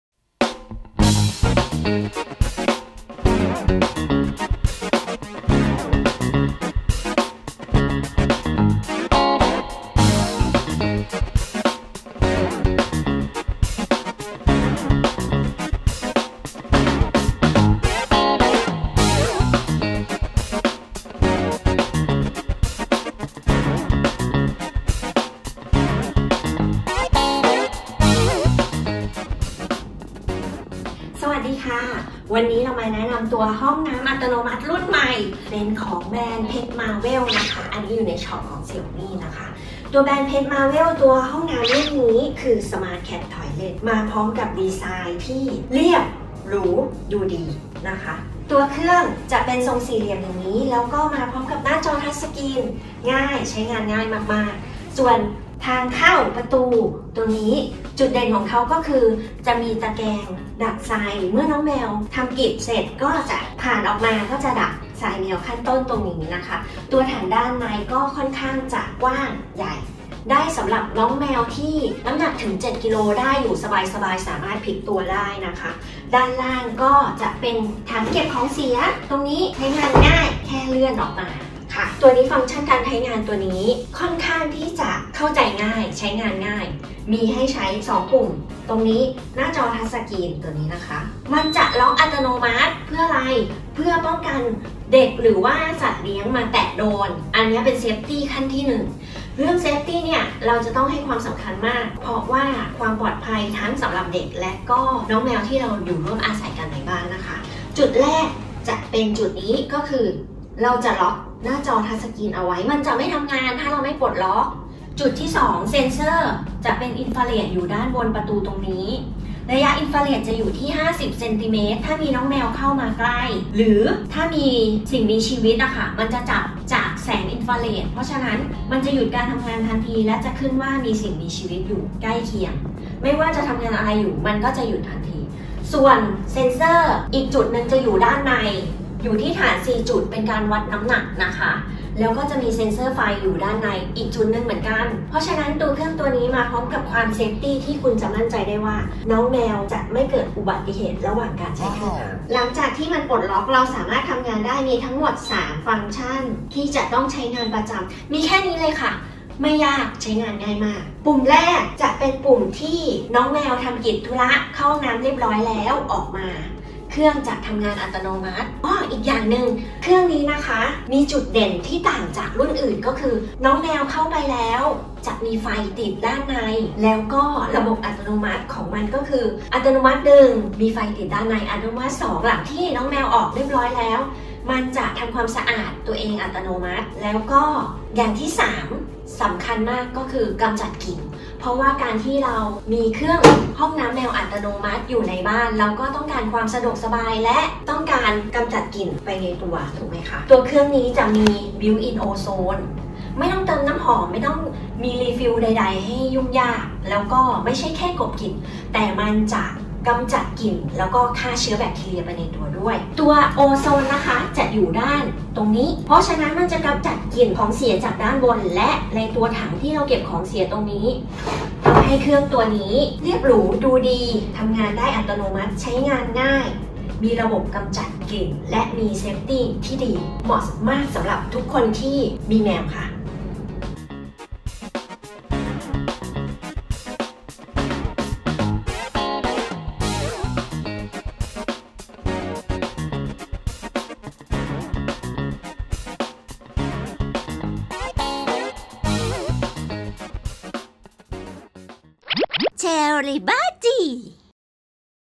รีวิวห้องน้ำแมวอัตโนมัติ PET MARVEL : EP1 เช็คสเปคเครื่อง ดียังไง ทำไมทาสแมวต้องมี!
-ห้องน้ำแมวอัตโนมัติ รุ่นใหม่ล่าสุด ที่ออกแบบด้วยทีม pet marvel
- เชื่อมต่อผ่านแอปมือถือได้ ตรวจดูการทำงานแบบ real-time ตลอด 24 ชม. มีระบบแจ้งเตือนสถานะต่างๆ
- มีเซนเซอร์รอบเครื่อง ทั้ง Double Optical Sensor ตรวจสอบว่าแมวอยู่ในเครื่องหรือไม่, Infrared Sensor เช็คภายในเครื่อง และ Weight Sensor ตรวจน้ำหนัก ทั้งหมดเพื่อความปลอดภัยของน้องแมว และข้อมูลที่ครบถ้วนทั้งหมด สำหรับผู้เลี้ยง
- มีระบบเททราย ภาดขยะอยู่ใต้เครื่อง ความจุถึง 8L แค่หยิบ รูดถุงแล้วนำไปทิ้ง
- มีเครื่องพ่นโอโซนในตัว รักษาความสะอาด และกลิ่นไปพร้อมๆ กัน
-หน้าจอที่มุมบนขวาของเครื่อง เป็นระบบ touch screen ใช้กดสั่งการทำงานเครื่องแบบ manual ได้

00:00 ไตเติ้ล
00:29 แนะนำตัวเครื่อง